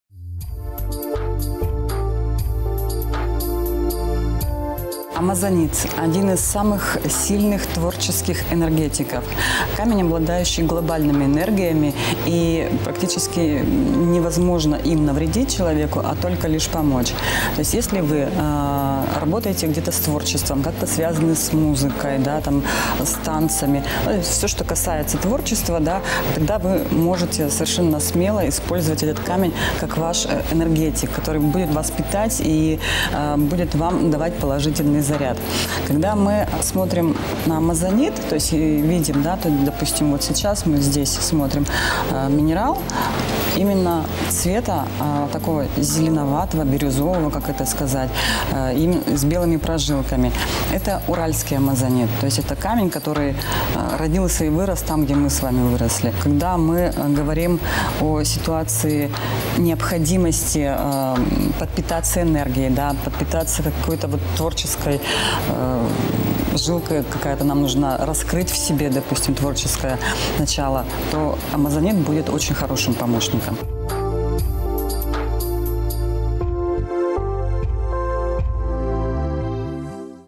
АМАЗОНИТ: КАМЕНЬ ВДОХНОВЕНИЯ И ТВОРЧЕСКОЙ ЭНЕРГИИ. Литотерапия. Светлана Миронова 05.10.23
Талисман творческих людей найден! Это камень амазонит. В России его впервые обнаружили в 1783 году на Южном Урале, в Ильменских горах. Минерал сразу понравился камнерезам. Он хорошо поддается обработке, имеет неповторимые цвет и текстуру. Как же амазонит раскрывает творческую энергетику, спросили у ювелирного стилиста и эксперта, консультанта по литотерапии Светланы Мироновой.